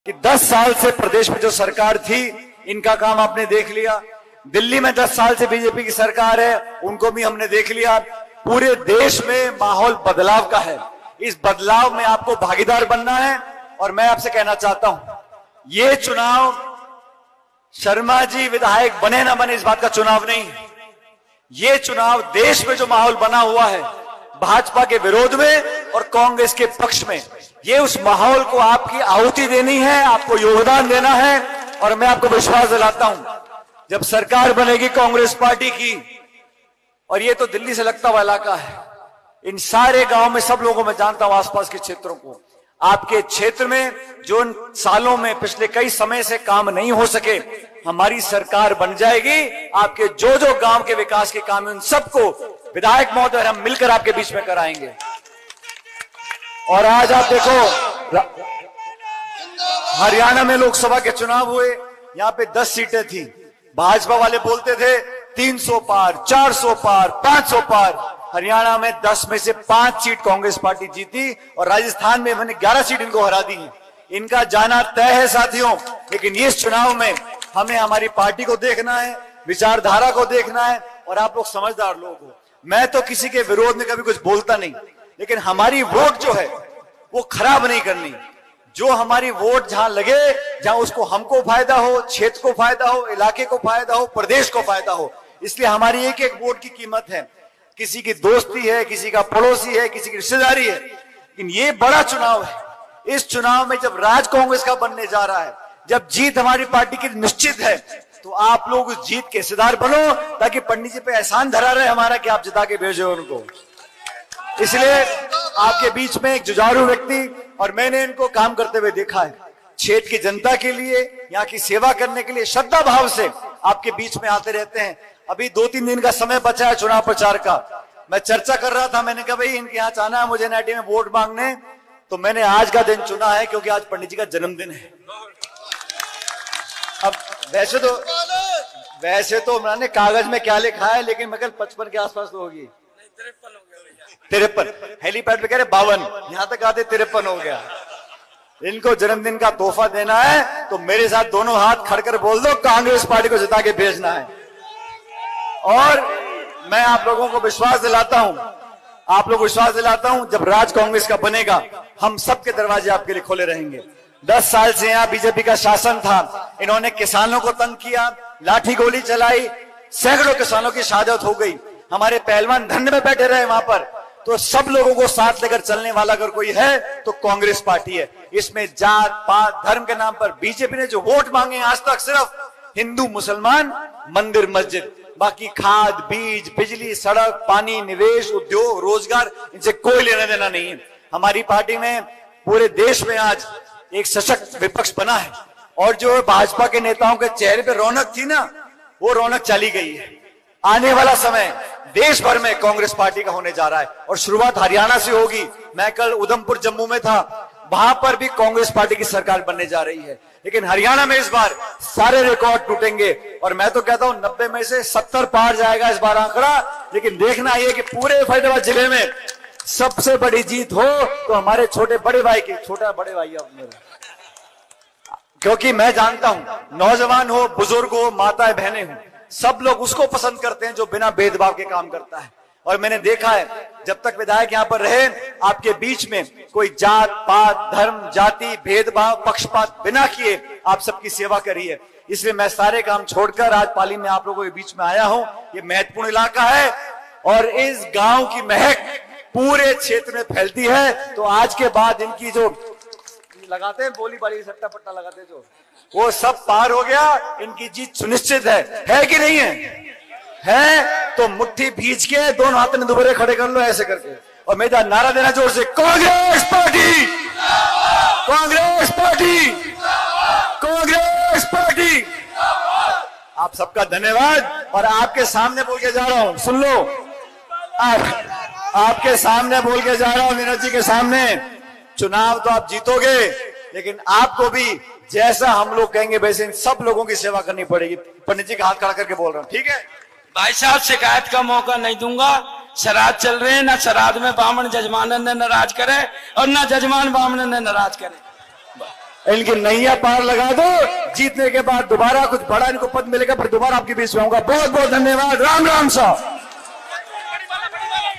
Sachin Pilot ने Haryana में दिया दमदार भाषण, उड़ जाएगी Khattar-Saini की नींद | Faridabad NIT
Haryana Vidhan Sabha Election में Rajasthan Congress के नेता और पूर्व उपमुख्यमंत्री Sachin Pilot ने Faridabad NIT Vidhan Sabha में दमदार भाषण दिया. उन्होंने कांग्रेस प्रत्याशी Neeraj Sharma के समर्थन में वोट मांगा. इस दौरान उन्होंने केंद्र की Narendra Modi सरकार पर तंज कसा. Manohar LaL Khattar और Nayab Singh Saini के लिए मुश्किलें बढ़ जाएगी.